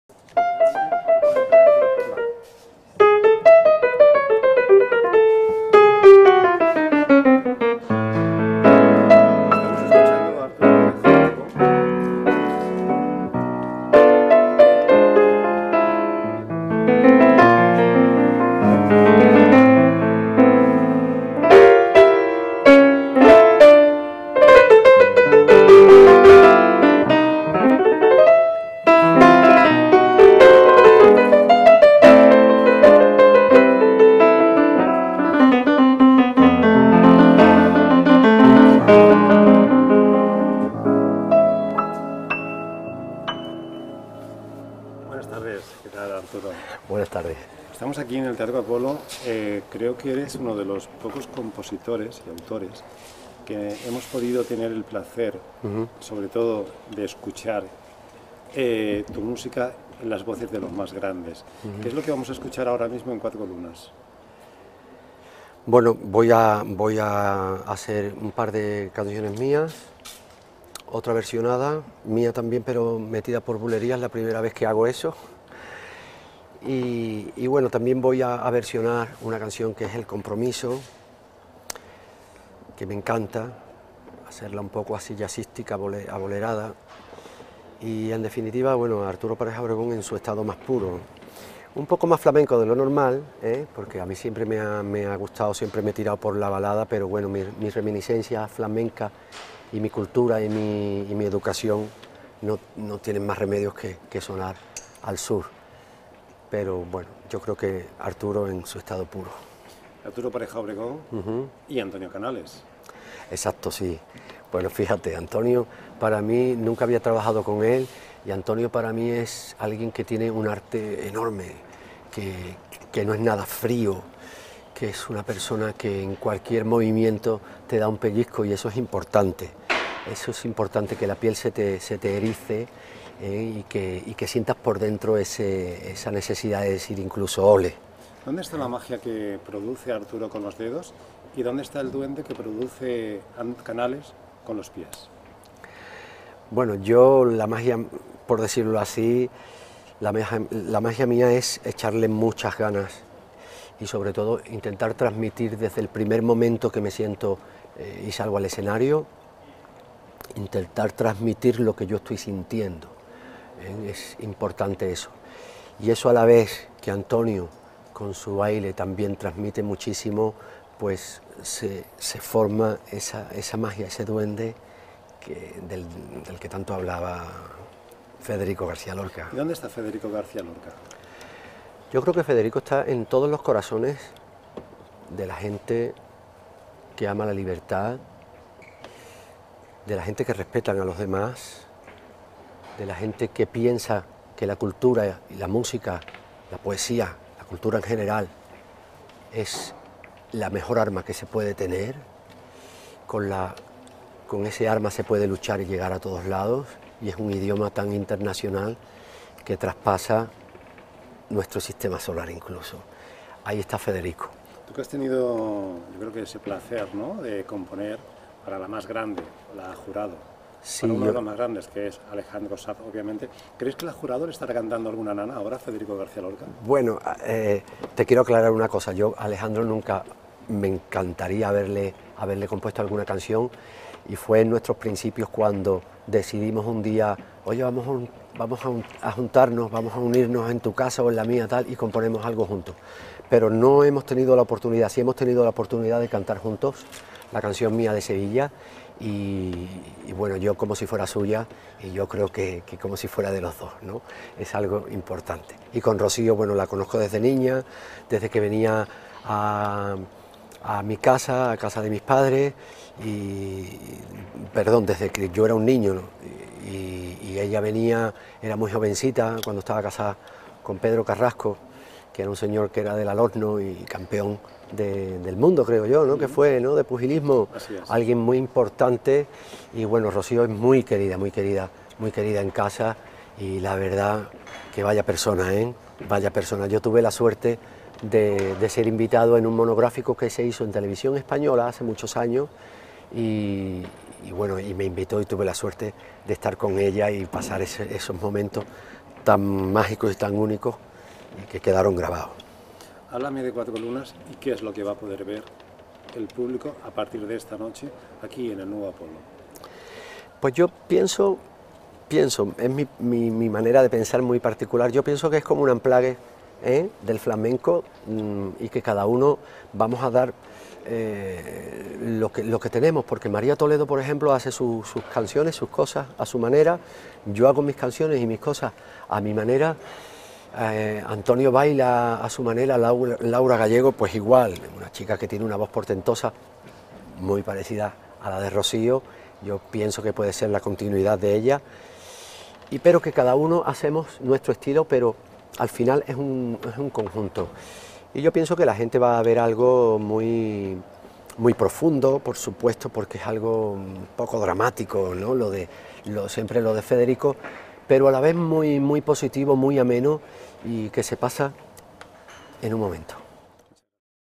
Entrevista Arturo Pareja Obregón - Cuatro Lunas - Teatro Nuevo Apolo
"Cuatro Lunas": Este musical está basado en el Juego y Teoría del Duende de Federico García Lorca, la base perfecta para un musical flamenco como el que dirige, con guión propio, Pilar Távora, responsable también de los textos. El baile va de la mano de Antonio Canales, a quien además se debe la coreografía, quien se va encontrando durante la función con las otras tres lunas: Diego Amador, Arturo Pareja Obregón y Laura Gallego. Su música al piano (en el caso de Amador y Pareja Obregón) y sus voces conducen al espectador a un mundo sonoro donde, por momentos, los sonidos negros de unos se unirán a los colores de otros.

"Cuatro Lunas" podrá verse del 10 al 21 de abril en el Teatro Nuevo Apolo de Madrid